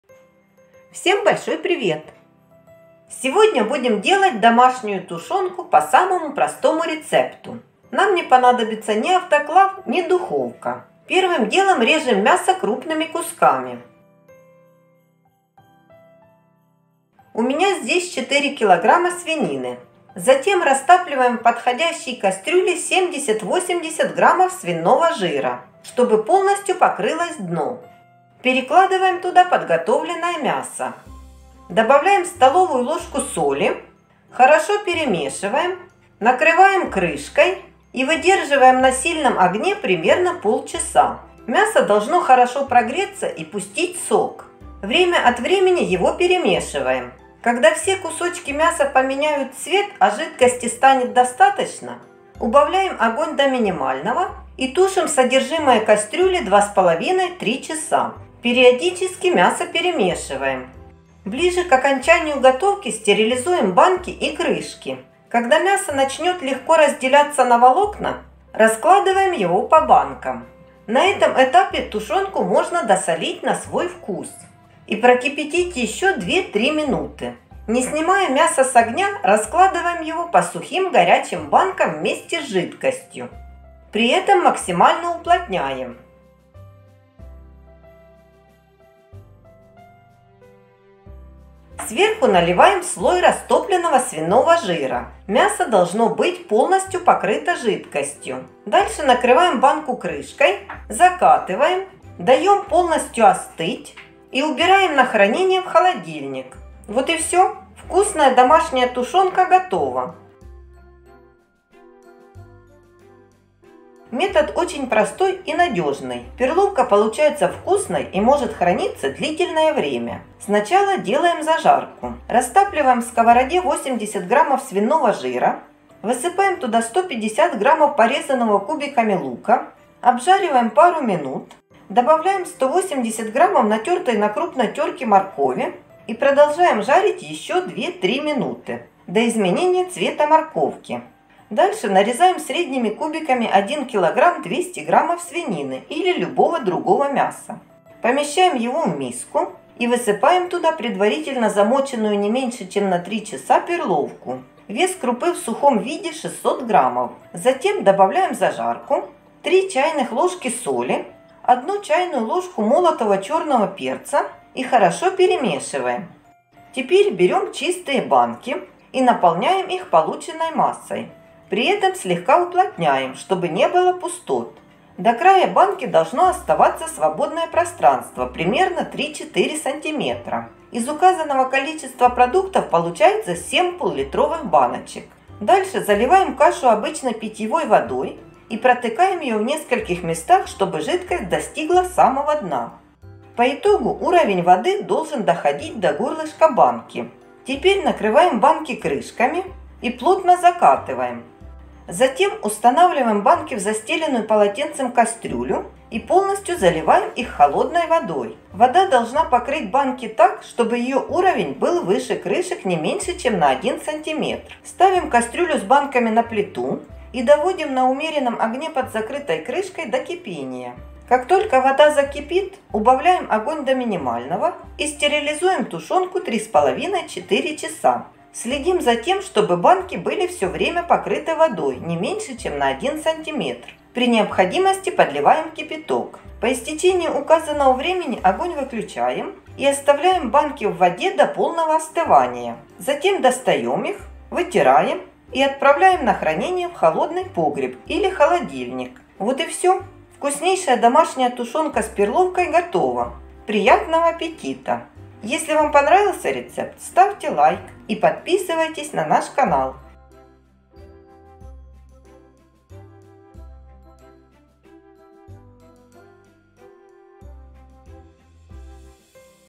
Домашняя ТУШЁНКА 2 рецепта как приготовить очень вкусное сочное МЯСО банках без автоклава и духовки
Тушёнка домашняя из свинины - 2 быстрых и простых рецепта для дома на любой вкус Максимальный срок годности такой тушенки 1,5- 2 года. А после открытия банки - 2-3 дня.

Для рецепта №1 (мясной вариант)

Ингредиенты:

* мясо (свинина) - 4 кг
* свиной жир - 100-120 гр
* соль - 25 гр (1 ст. л.) или по вкусу

Для рецепта №2 (с добавлением перловой крупы)

Ингредиенты:

* мясо - 1.2 кг
* перловка - 600 гр
* свиной жир - 80 гр
* черный перец - 3 гр (1 ч. л.)
* лук - 150 гр (1-2) шт
* морковь - 180 гр (1 шт крупная)
* соль - 18 гр (3 ч. л.)

Выход: 7 баночек емкостью 0,5 л

👉 ‼️ВАЖНО‼️